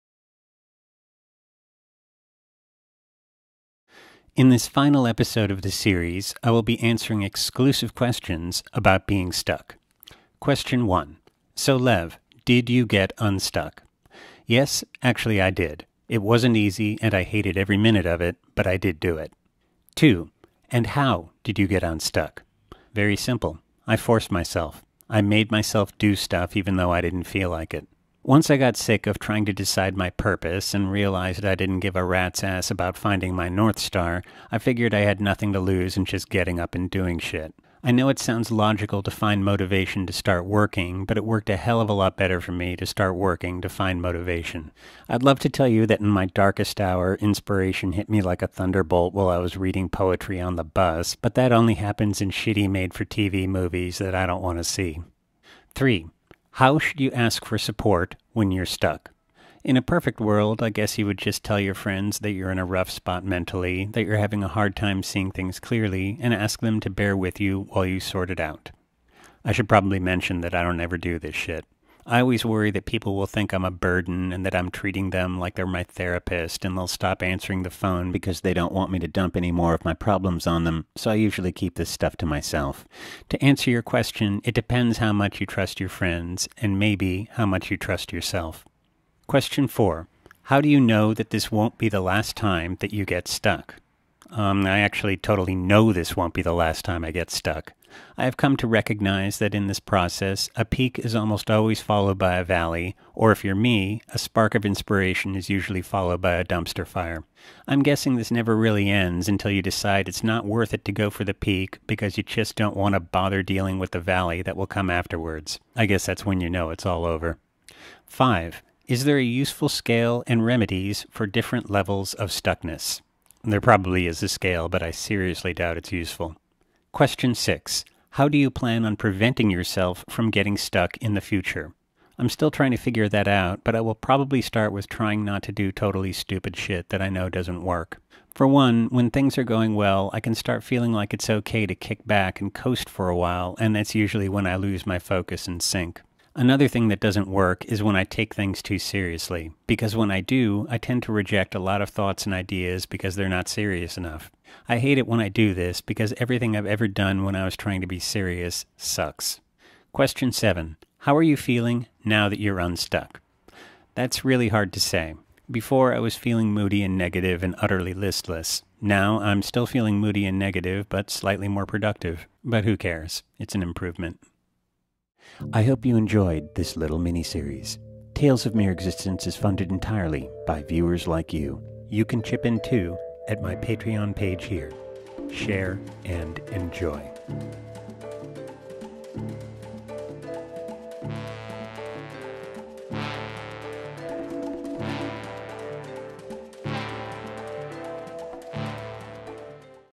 "STUCK: Episode 5" Tales Of Mere Existence
A Question and Answer session about being Stuck, and getting Un-Stuck.

(Fifth and final episode of a Mini Series about being Stuck, and what it's like to try to get Un-Stuck.)

On my channel I share humorous, detailed stories from my life that I draw and animate into short videos.